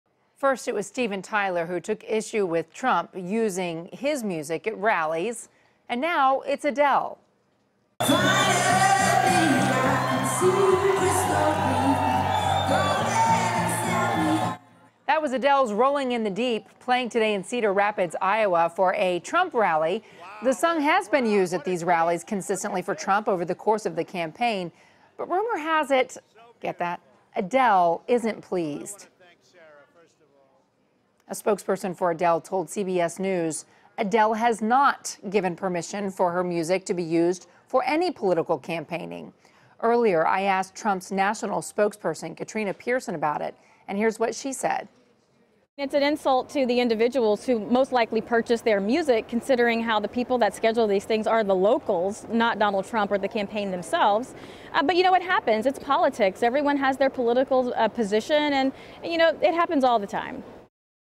Adele wants Donald Trump to stop using her music
Singer Adele has one message for GOP frontrunner Donald Trump: Stop using my music at your rallies. CBSN's Contessa Brewer has more.